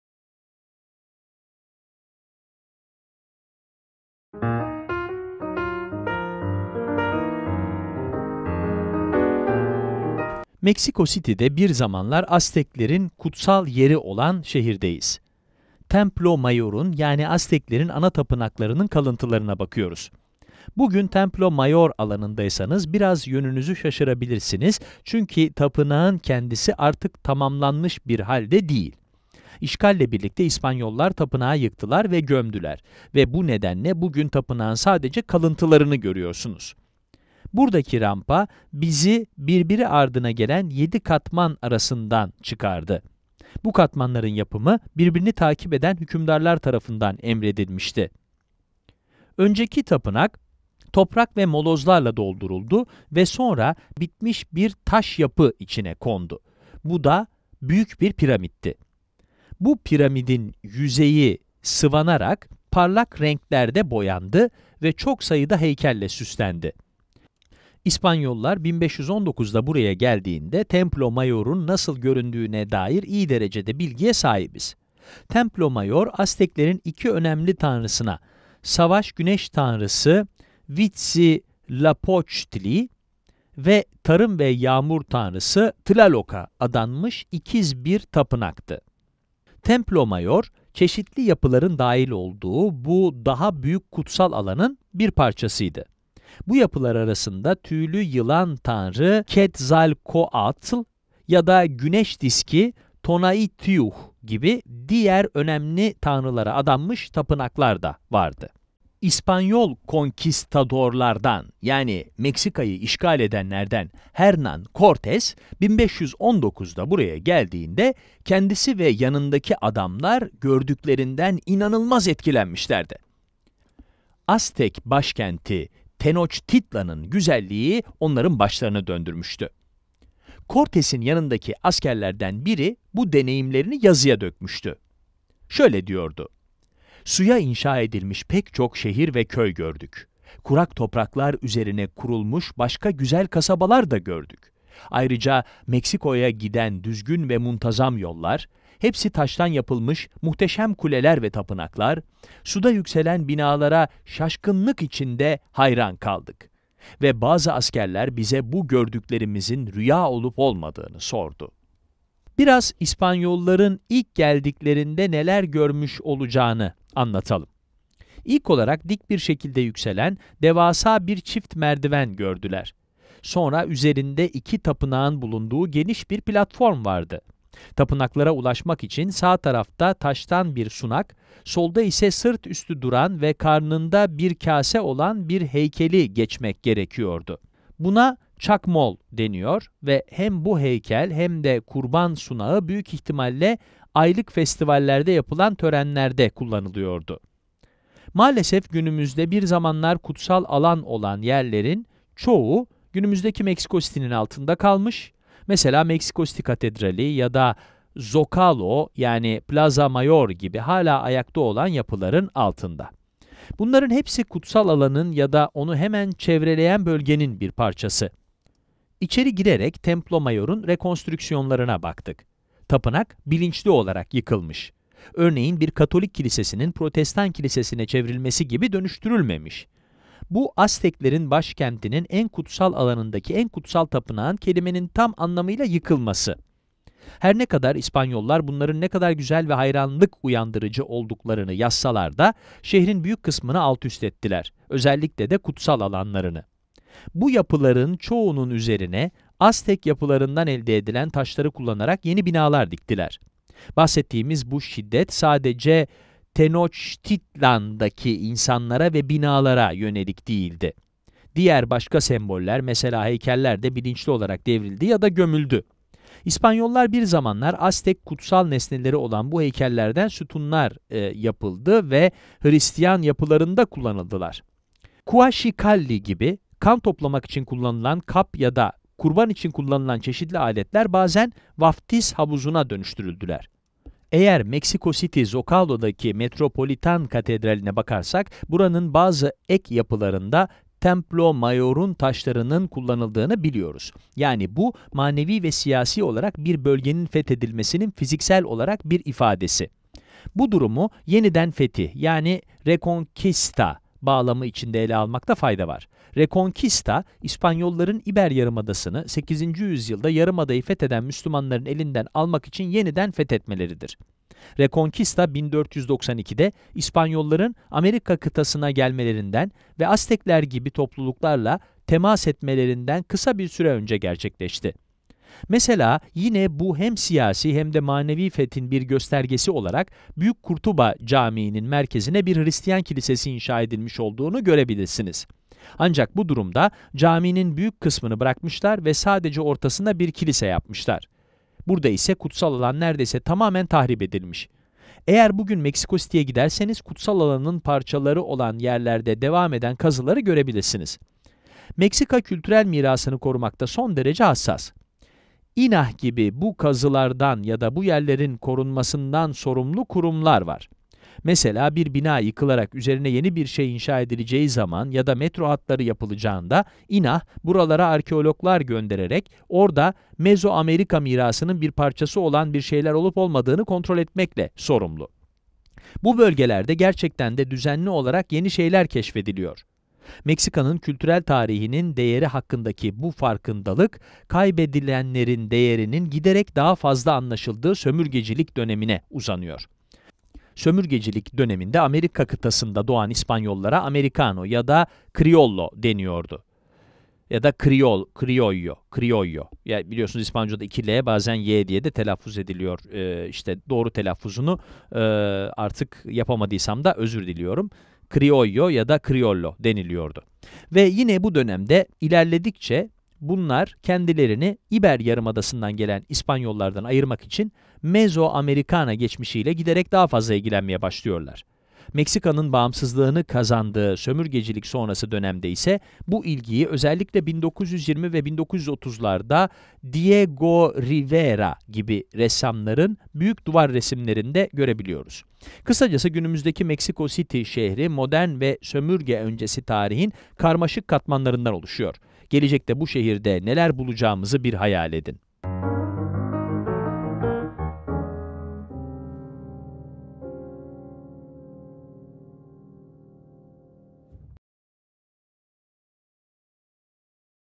Aztek Geçmişini Gün Işığına Çıkarmak: Templo Mayor'un Yıkılması (Sanat Tarihi)
Unearthing the Aztec past, the destruction of the Templo Mayor